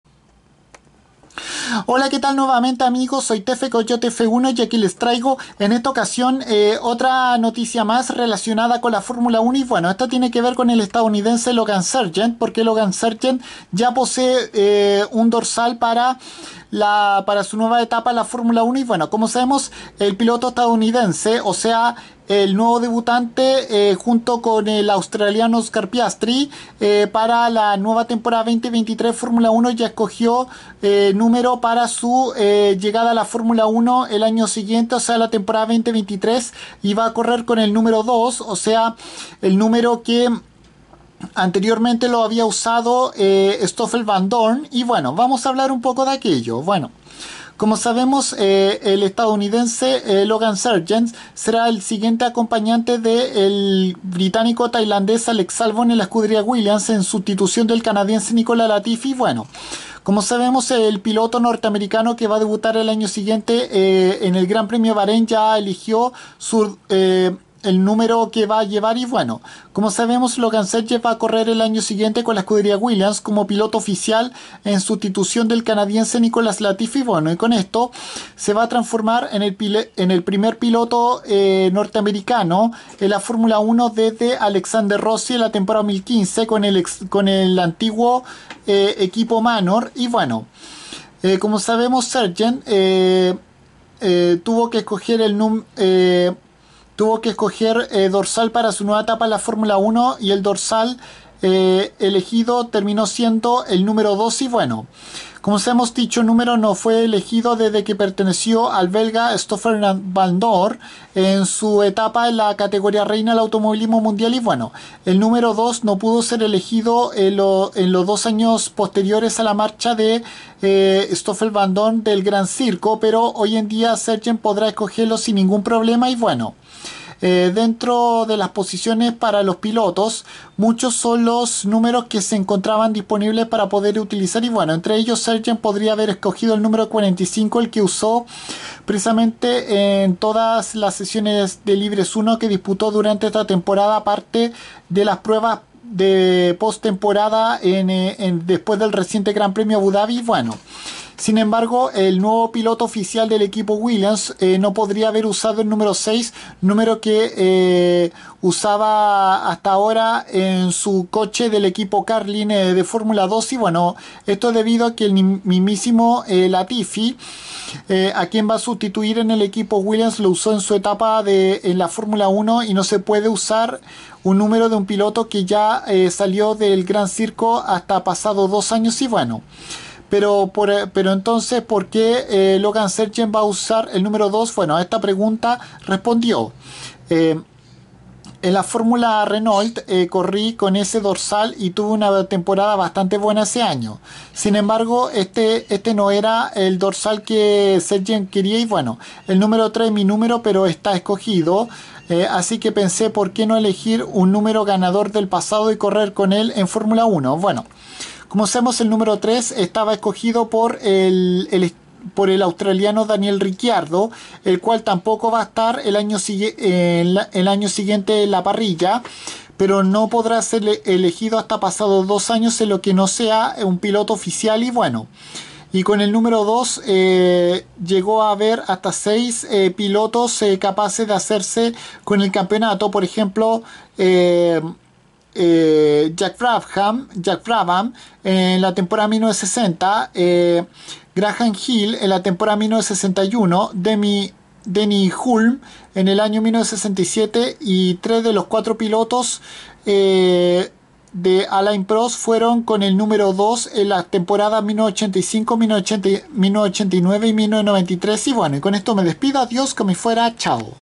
Logan Sargeant escoge el dorsal #2 para su debut en la F1 en 2023
El piloto norteamericano nuevo piloto oficial de Williams escogió precisamente el mismo número que utilizó hasta 2018 el belga Stoffel Vandoorne para la nueva Temporada 2023 de la Fórmula 1